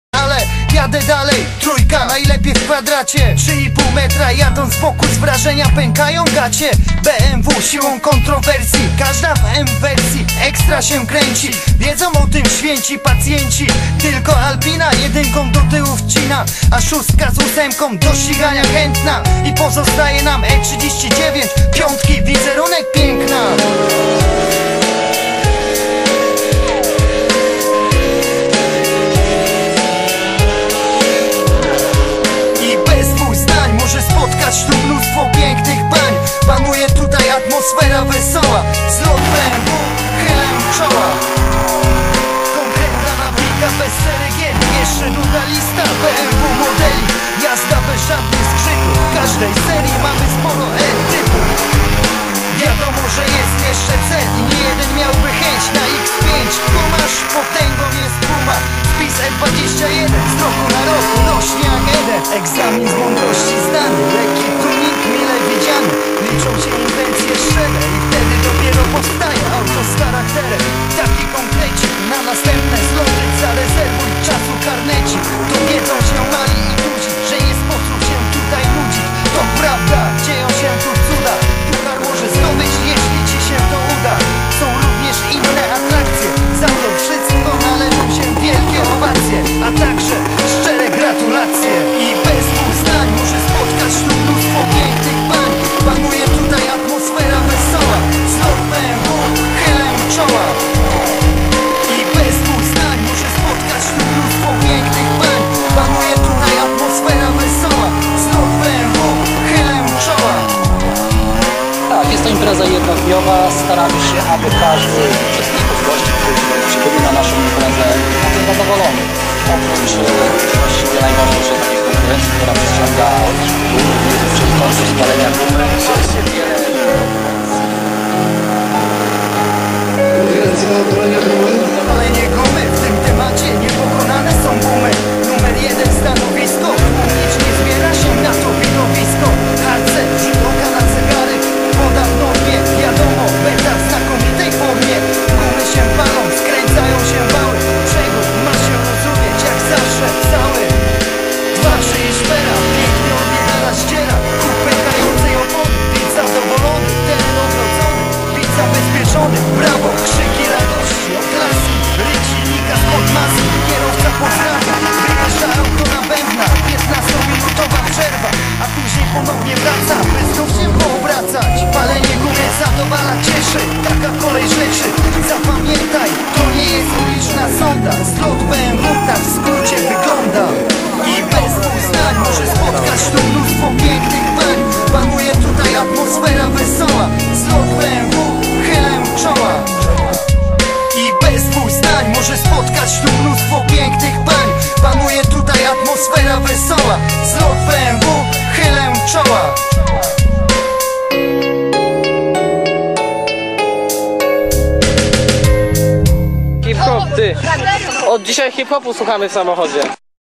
Palenie gumy "Langoś " majówka BMW 2010
Tak sie pali gumę na 5 biegu :)
 Zdjecia i film z palenia gumy

Moim zdaniem najlepsze palenie gumy majówki
Pozdro dla posiadacza tej E30 i CGD Klub Golub- Dobrzyń